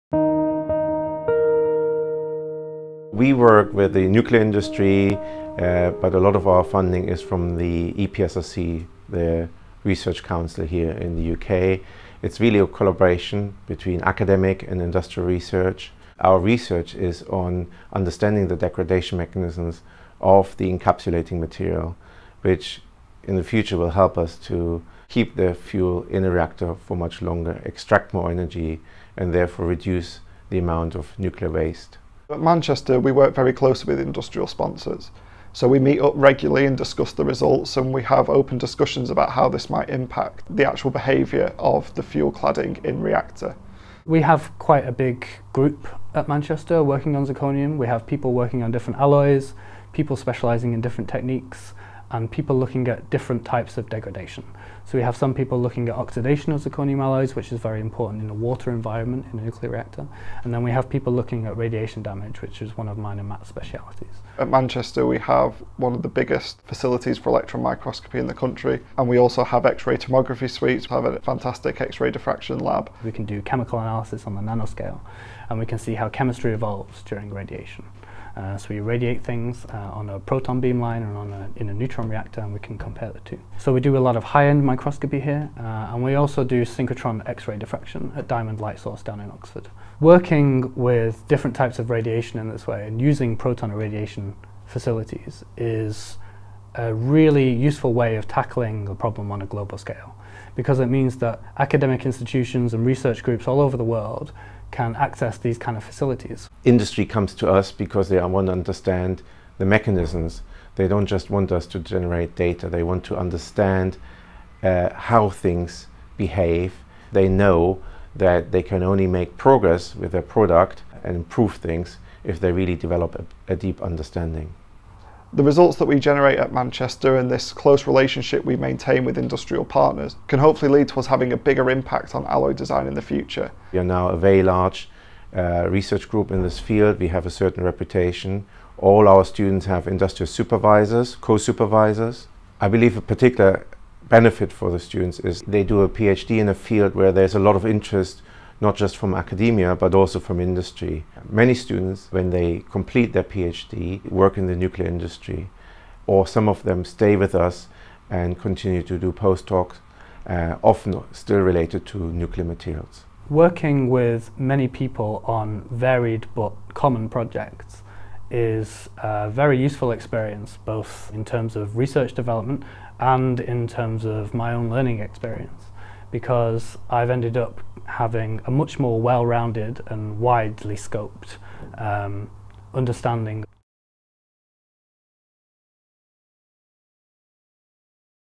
Zirconium Research Impact
CDT students from the Zirconium Group at the University of Manchester describe their research with their supervisor, Prof Michael Preuss.

Find out more about the EPSRC Centre for Doctoral Training in Advanced Metallic Systems